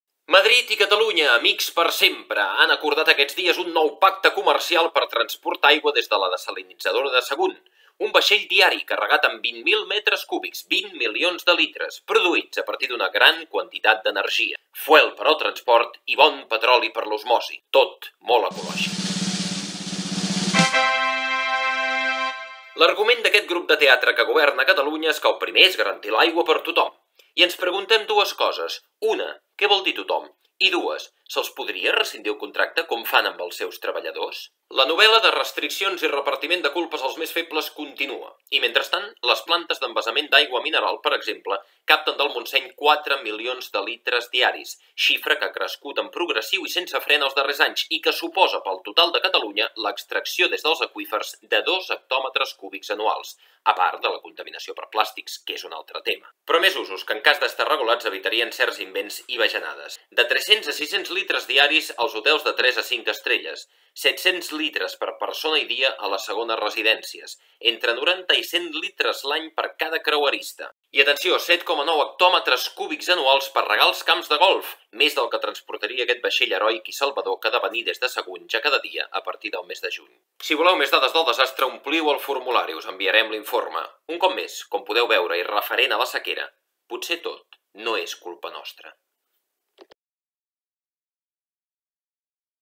EMERGÈNCIA per SEQUERA 4 | Indústria
Madrid i Catalunya, amics per sempre, han acordat aquests dies un nou pacte comercial per transportar aigua des de la dessalinitzadora de Sagunt.
Un vaixell diari carregat amb 20.000 metres cúbics, 20 milions de litres, produïts a partir d’una gran quantitat d’energia. Fuel per al transport i bon petroli per l’osmosi, tot molt ecològic.
L’argument d’aquest grup de teatre que governa Catalunya és que el primer és garantir l’aigua per tothom. I ens preguntem dues coses:
1: Què vol dir tothom? 2: Han passat el període de prova aquesta gent? La novel·la diària de restriccions i repartiment de culpes als més febles continua. I mentrestant les plantes d’envasament d’aigua mineral, per exemple, capten del Montseny 4 milions de litres diaris, xifra que ha crescut en progressiu i sense fre en els darrers anys i que suposa, pel total de Catalunya, l’extracció des dels aqüífers de 2 hectòmetres cúbics anuals.
A part de la contaminació per plàstics que és un altre tema. Però més usos, que en cas d’estar regulats, evitarien invents i bajanades: De 300 a 600 litres diaris als hotels de 3 a 5 estrelles. 700 litres per persona i dia a les segones residències 90 a 100 litres l’any per cada creuerista. I atenció: 7,9 hectòmetres cúbics anuals per regar els camps de golf, més dels que transportaria aquest vaixellheroic i salvador que ha de venir des de Sagunt, ja cada dia, a partir del mes de juny. Si voleu més dades del desastre ompliu el formulari
Us enviarem l’informe.

Un cop més, com podeu veure, i referent a la sequera, potser tot NO és culpa nostra.